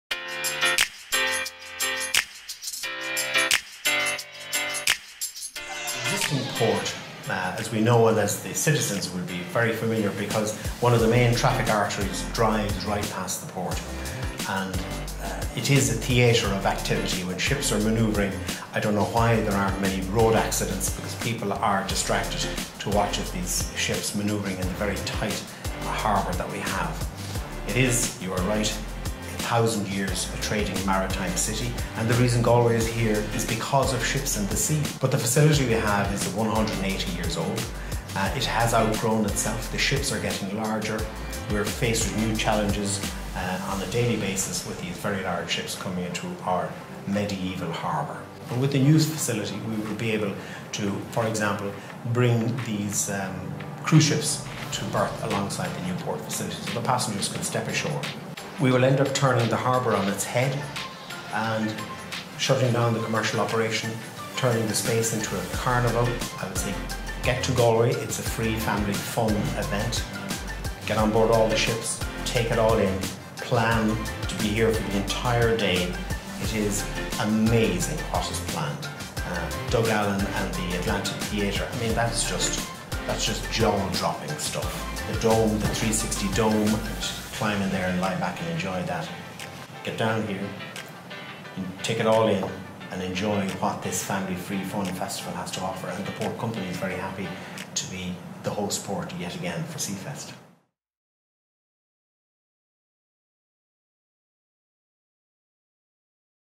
SeaFest 2018 - Captain Brian Sheridan, Port of Galway
Captain Brian Sheridan of the Port of Galway, talks about hosting Ireland's biggest maritime festival, SeaFest 2018.